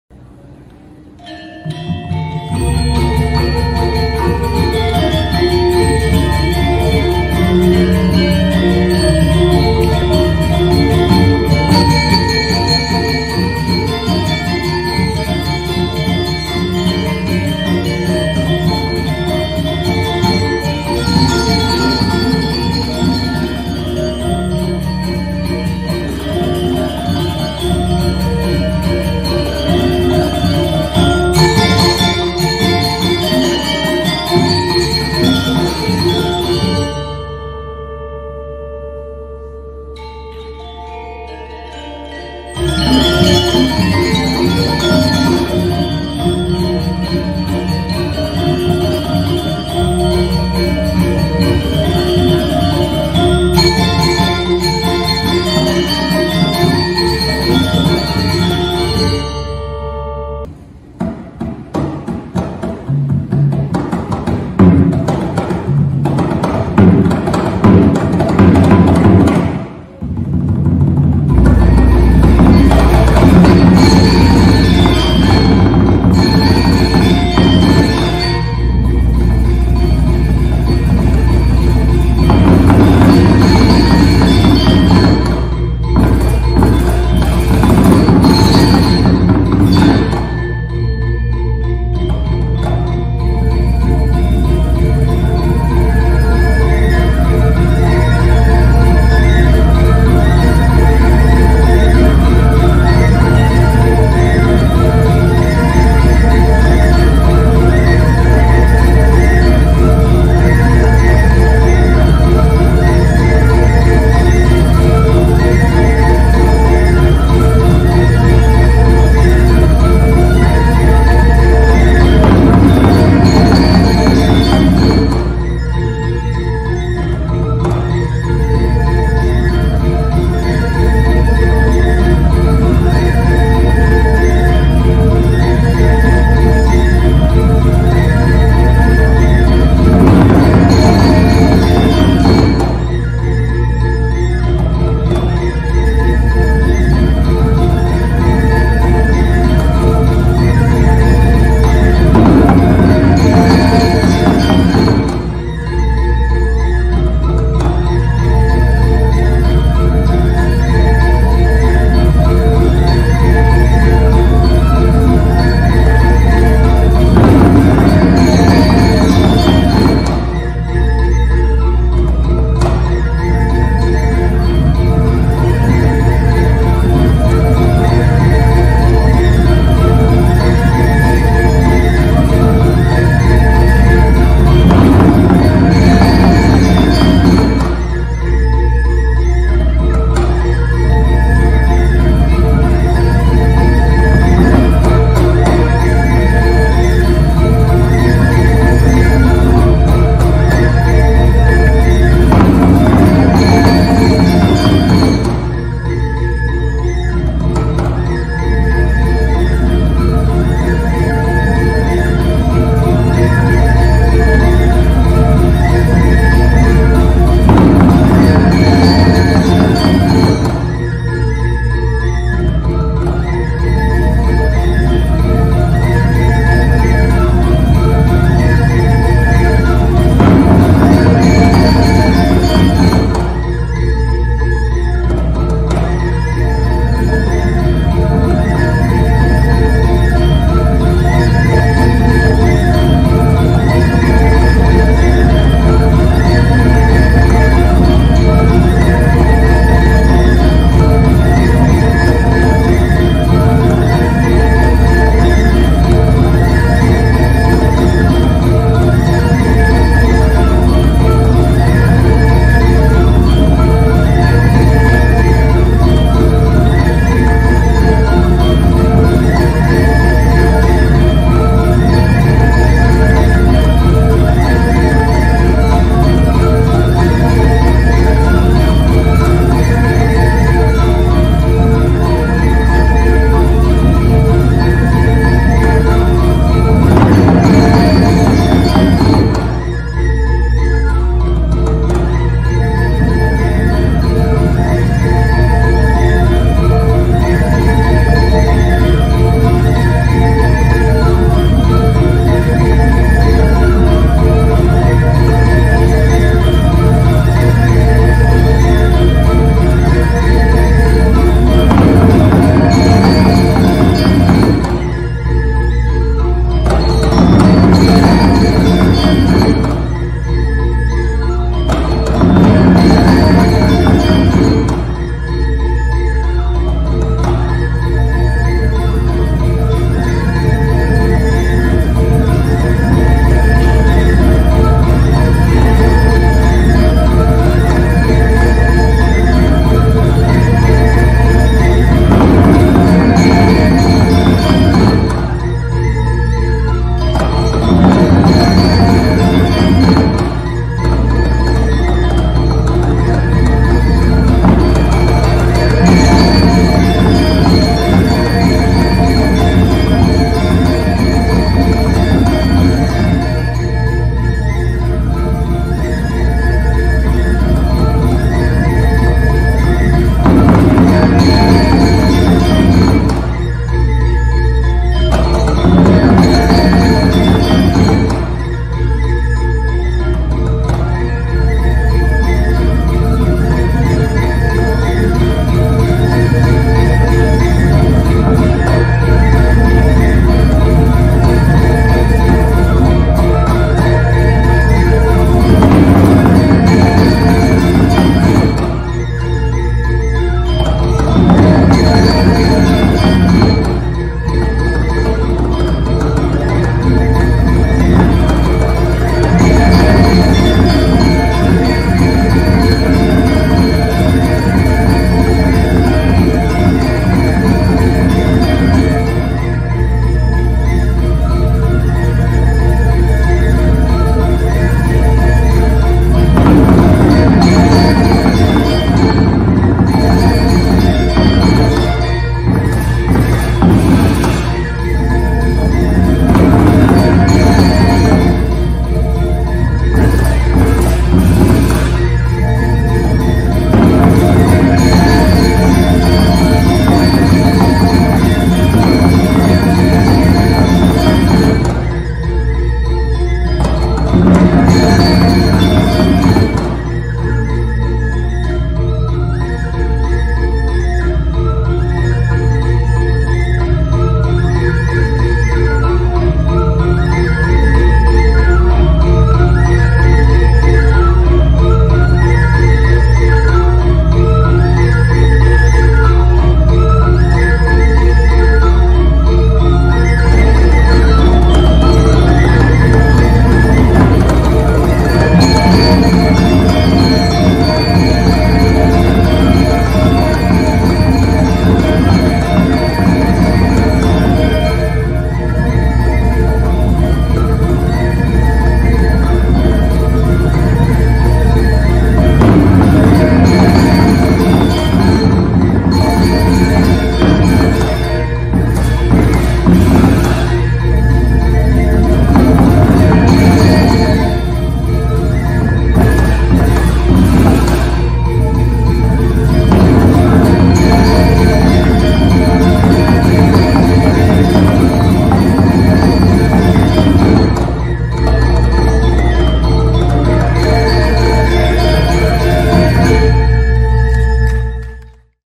TARI TOPENG BALI PENTAS DI PKB 2021 (Topeng Prembon)
Tari Topeng Bali yang telah pentas di Pesta Kesenian Bali PKB 2021 sangat mengesankan bagi pentonton. Tari Topeng Bali ini merupakan tari pembukaandari sebuah pentas Topeng Prembon Lebur Guntur.